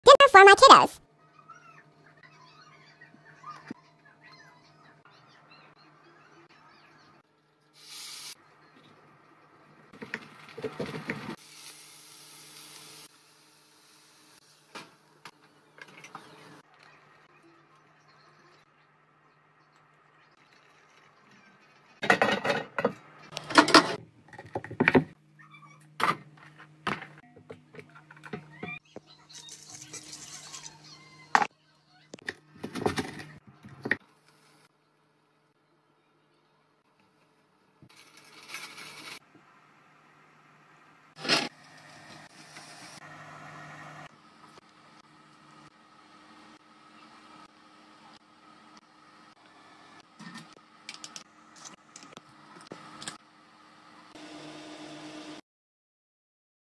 Delicious Pot Roast Sunday Dinner For My Kiddos
Sunday Dinner For My Kiddos

Beef Chuck Pot Roast Recipe⬇️

Beef broth
better than bullion
salt
Green bell pepper
Carrots
Onion power
Garlic powder
Lipton onion soup mix
Onion
Potatoes
Beef chuck roast
parsley

Lawry’s all purpose low sodium seasoning salt

Cooked roast in oven for two hours, and 30 minutes after two hours have passed, then add potatoes and carrots. I took a cup of water, added 2 tablespoons of flour, mixed until dissolved, poured it in the roast to make my gravy thicker

Everything I use in the kitchen link is in bio

Hope you all have a blessed and safe night 🌚🙏🏾😇♥️